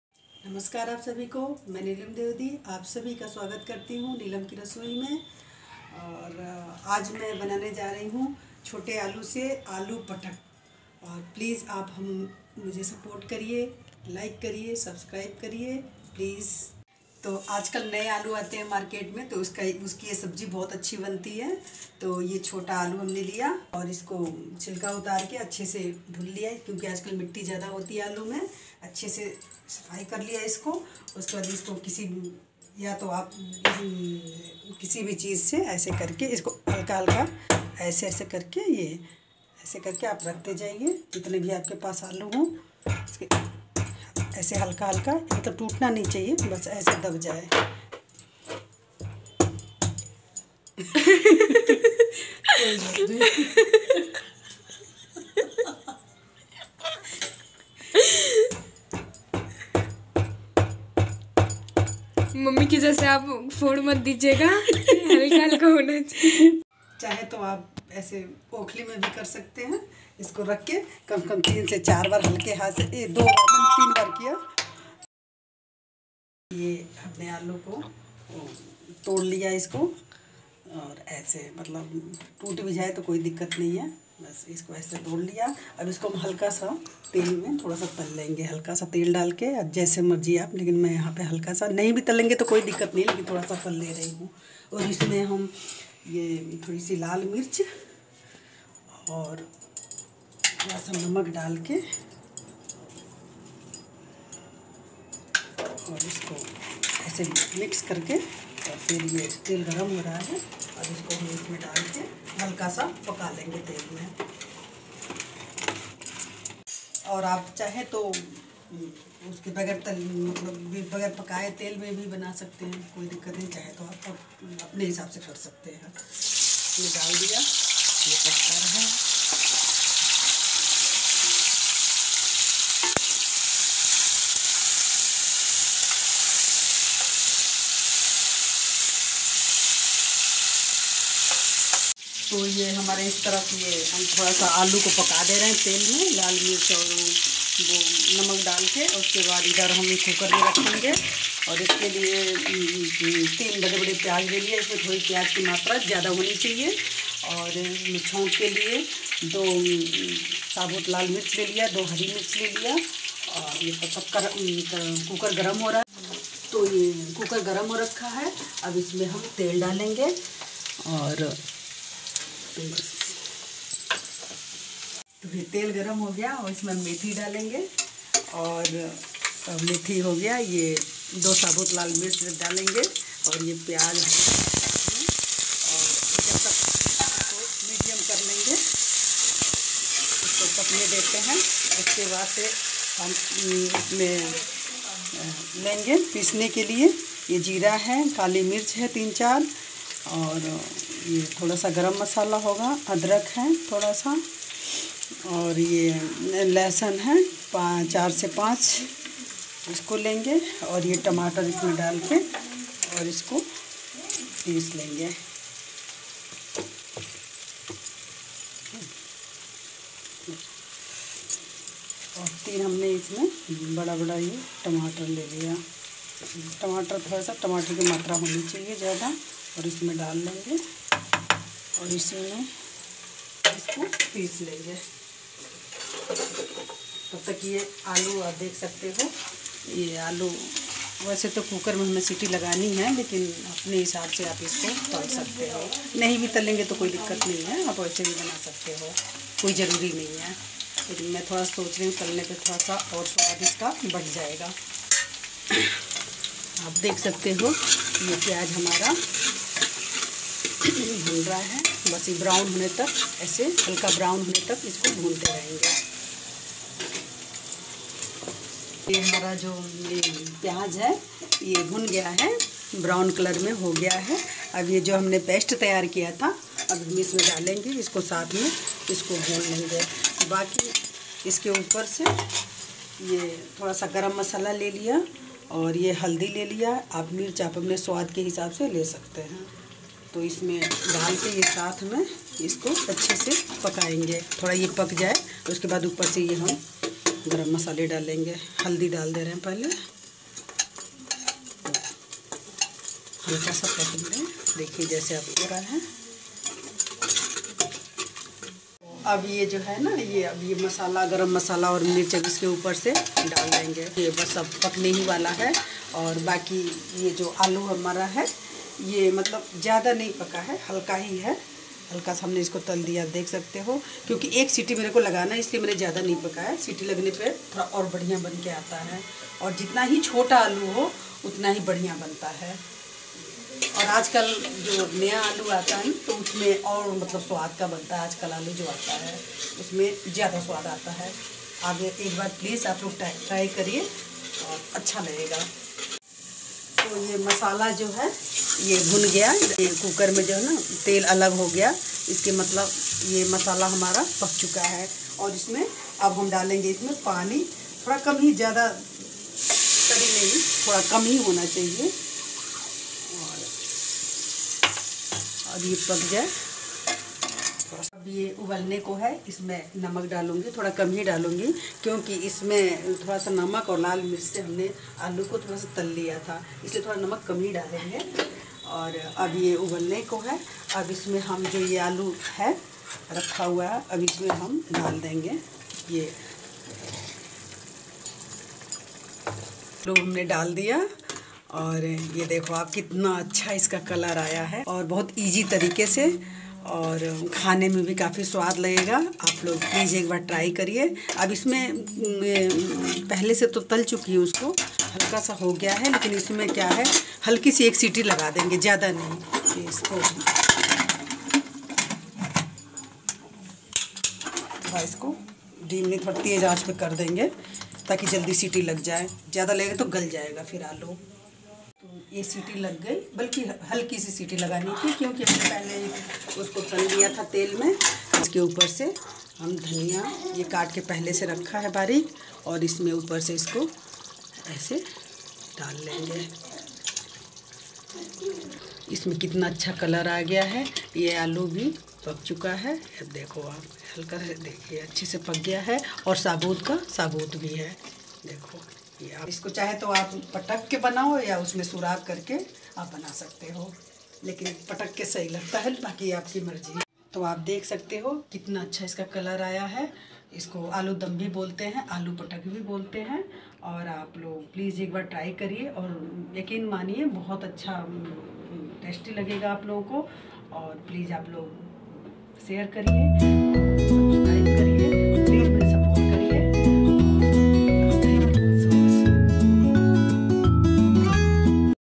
तीखा चटपटा दम आलू की आसान और एकदम स्वादिष्ट विधि | Dum aloo recipe| Aloo patak Recipe
Namaste dosto🙏, aaj mein aapke liye Asaan aur swadisht ( Aloo dum/aloo pataka) ki recipe le kar aayi hu. Asha karti hu ki aapko ye Video pasand aayegi.